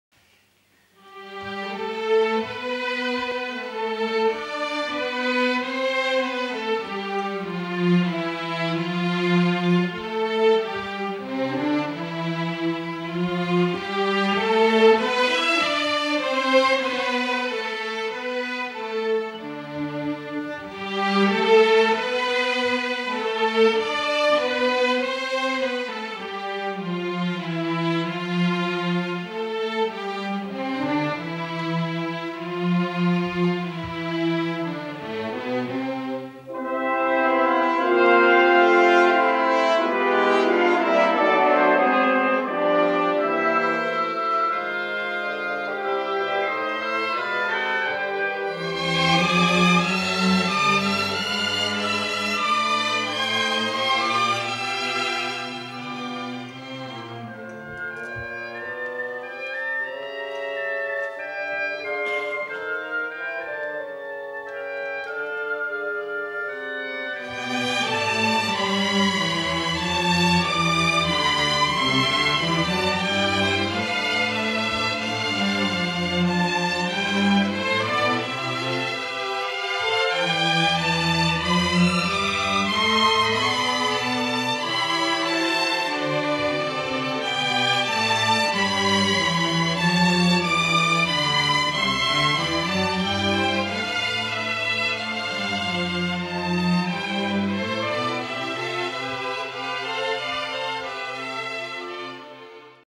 Concert simfonic
Un clasic al muzicii noastre, Ion Dumitrescu a fost compozitor, profesor, muzicolog, dirijor, scriitor, membru corespondent al Institutului Franţei - Academie des Beaux - Arts şi al Academiei "Tiberina" din Roma.